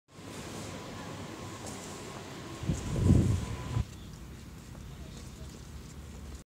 "незахищена зварка" на будівництві ТРЦ біля станції Київського метро Героїв Дніпра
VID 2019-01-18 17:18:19

незащищенная сварка на стройке ТРЦ возле станции Киевского метро Героев Днепра